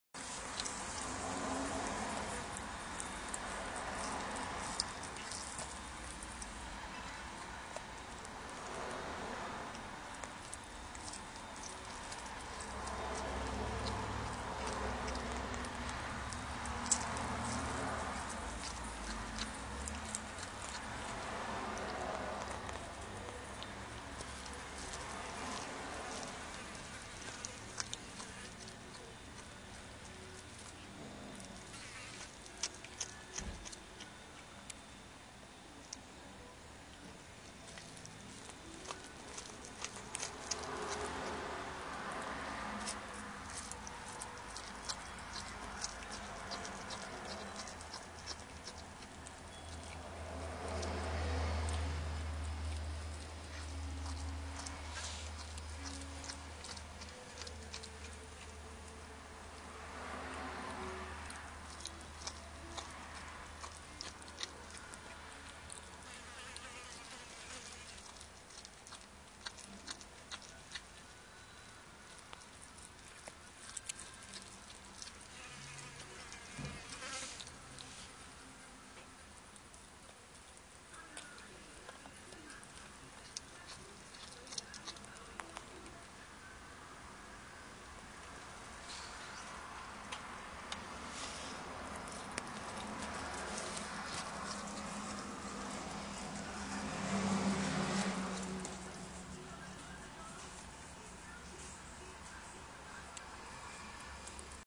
Meat Eating Machine.
Watching LightFoot Enjoy His Ham.